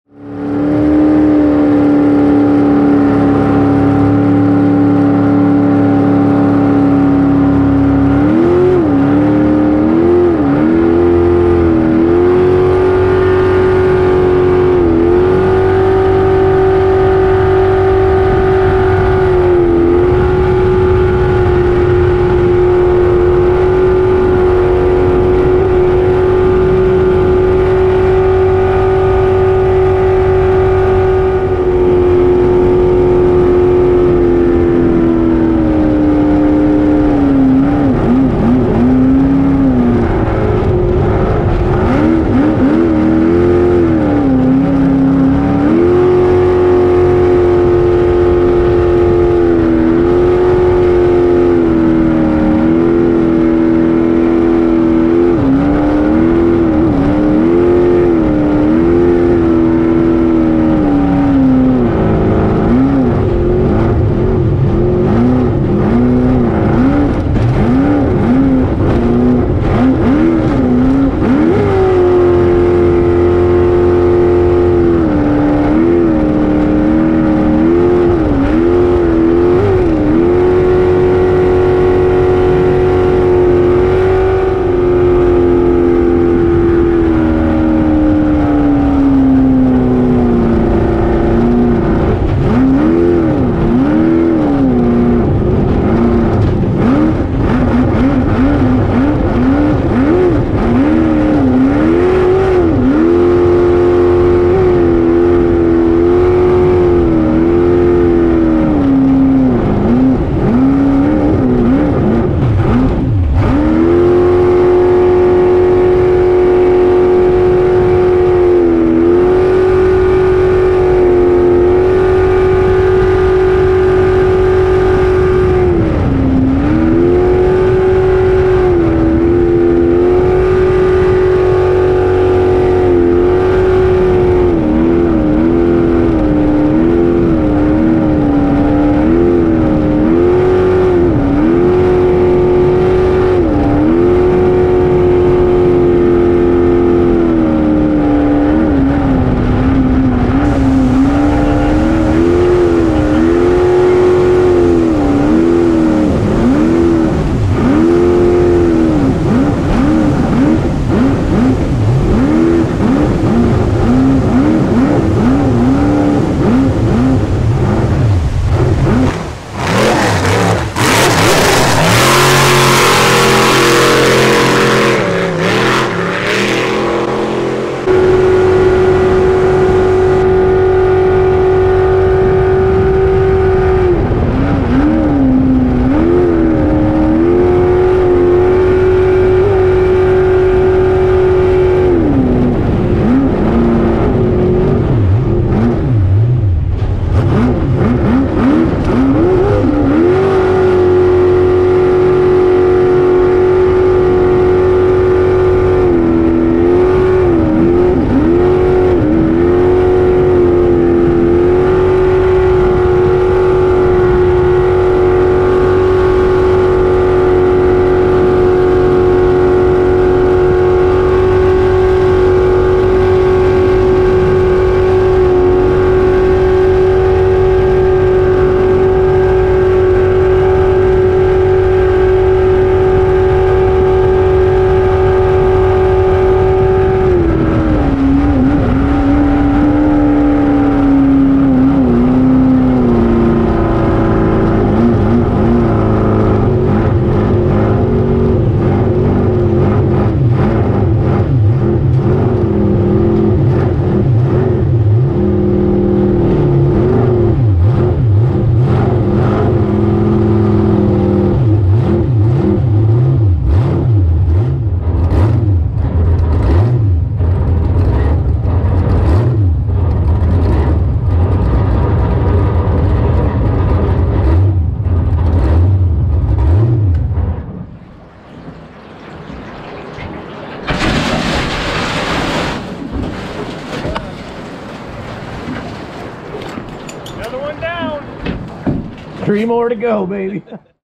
A208 Always Satisfied. 2022 Salmon River Jet Boat Race
Jake Barney and Gage Fiamengo Race the Salmon River 2022